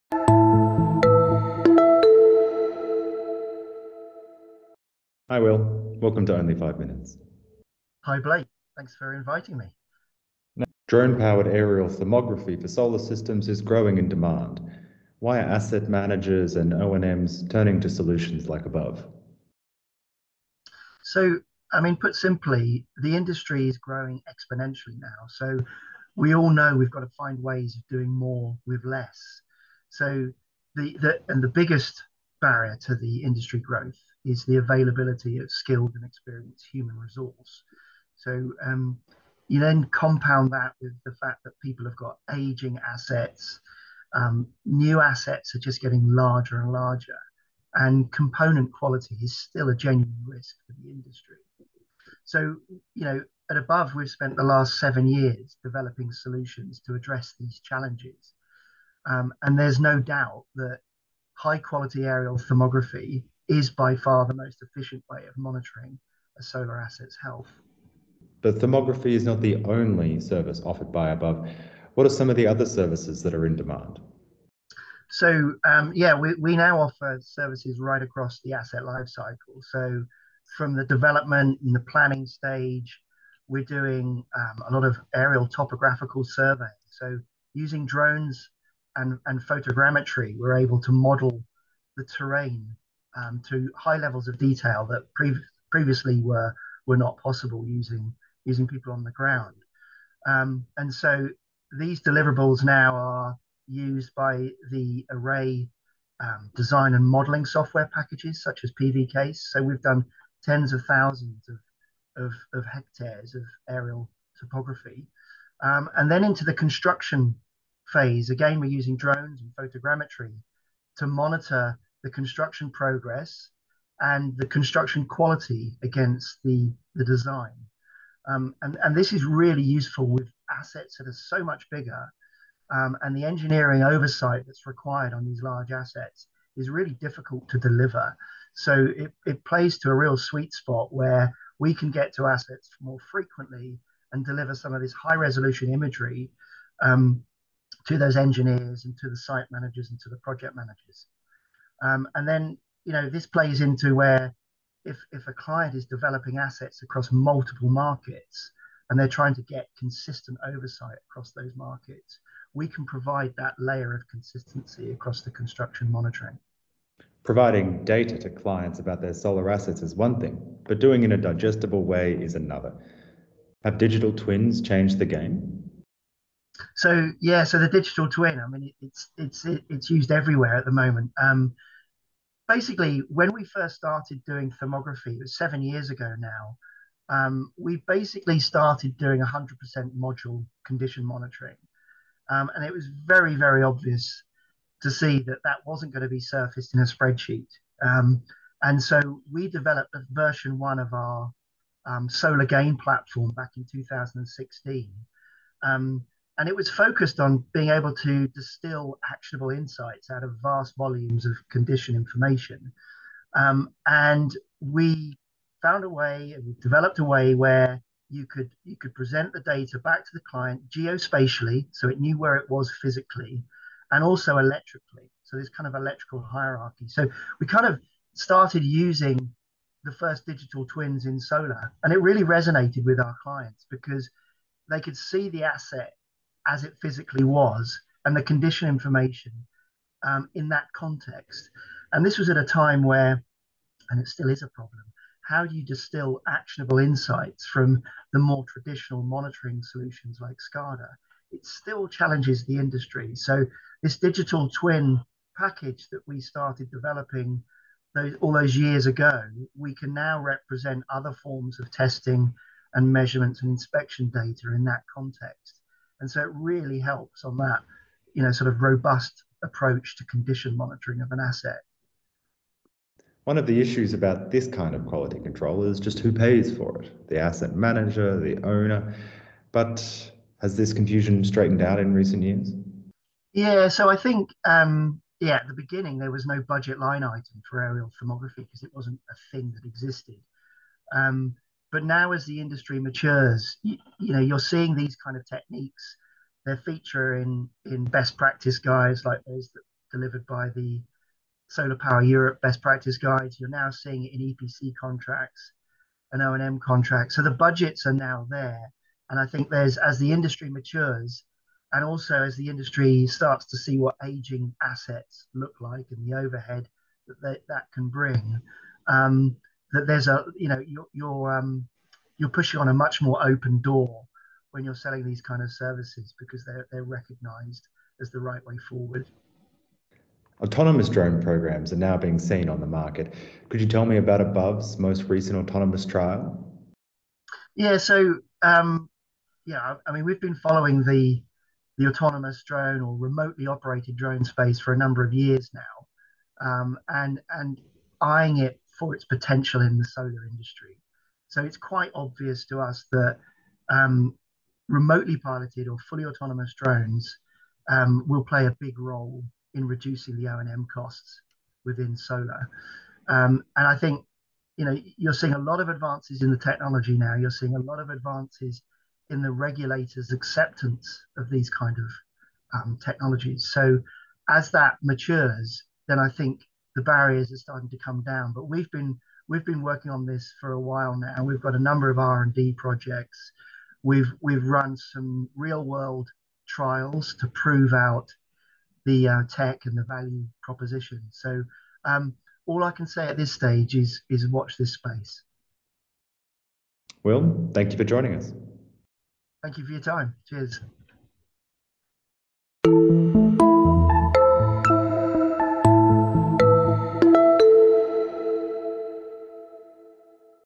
Above and beyond - Only5mins! with Will Hitchcock
United Kingdom-based Above’s founder and CEO Will Hitchcock sits down with pv magazine to discuss the solar industry’s growing demand for drone-based aerial inspection and data analysis, the game changing utility of digital twins, and what the future holds for autonomous drone programs.